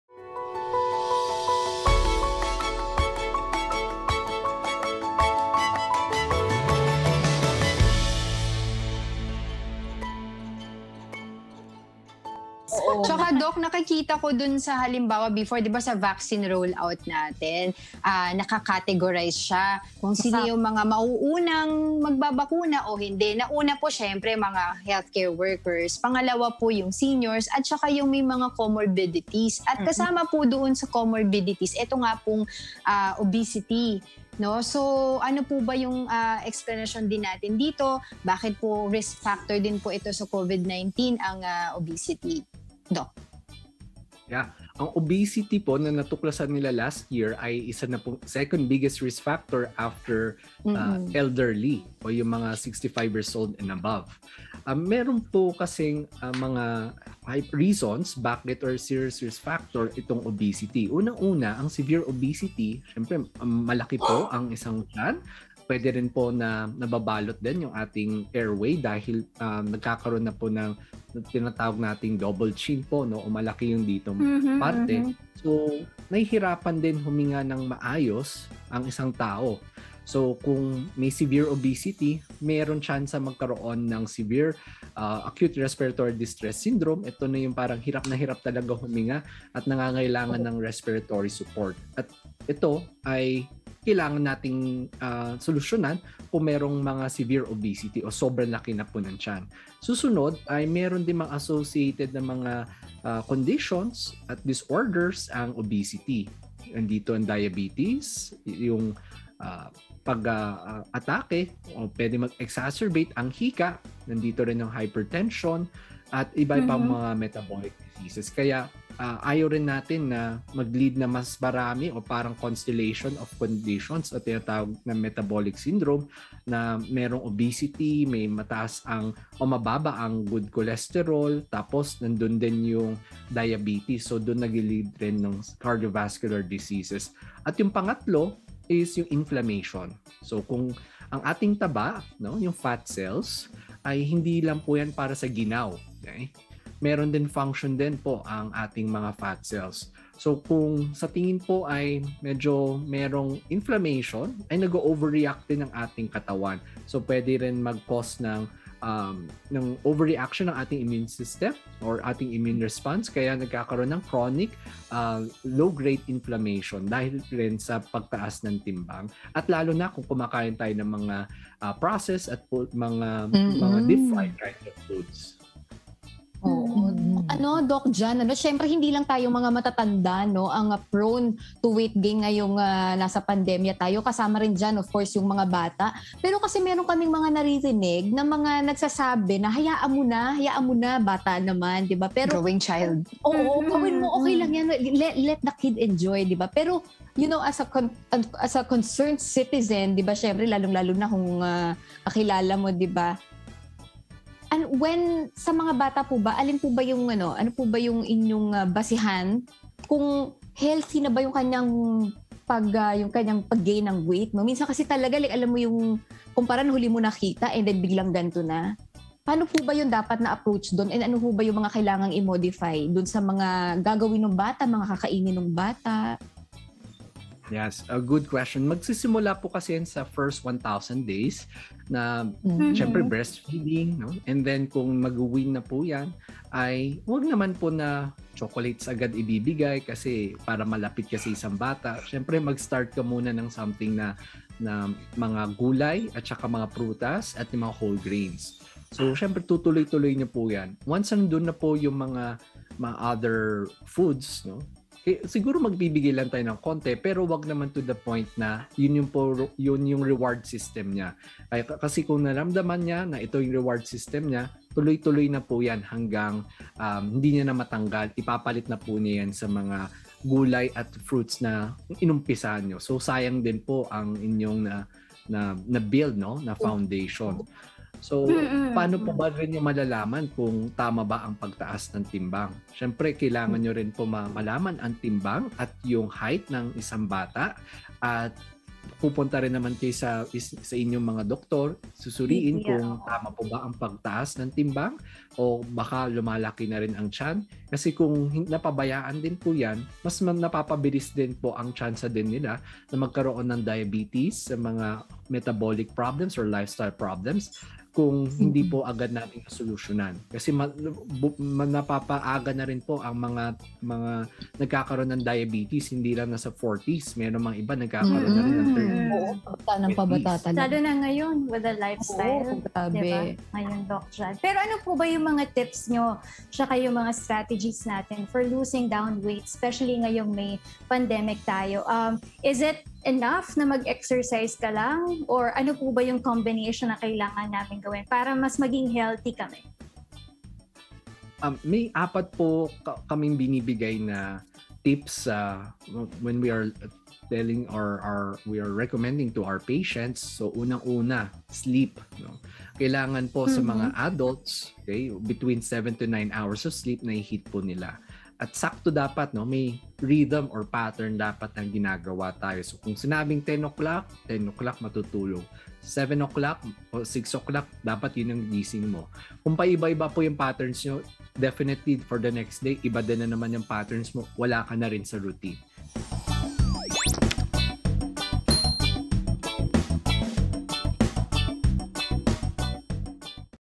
Weight Management: How to Stay Active during Pandemic? (Part 2) | Usapang Pangkalusugan
Bakit risk factor sa Covid-19 ang obesity? 😳